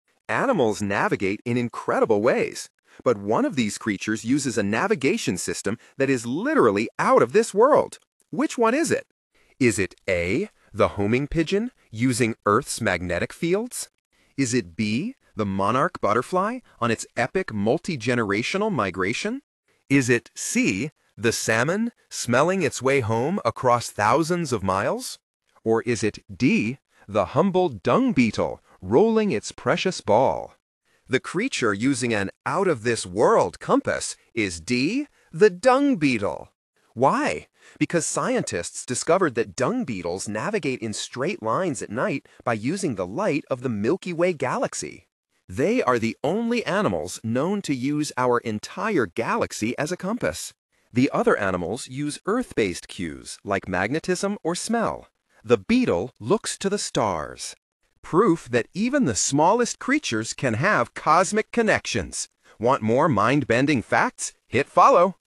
Which Tiny Animal Uses the GALAXY as a Compass?
One of these creatures navigates using a method that is literally out of this world. Can you guess which one uses the Milky Way?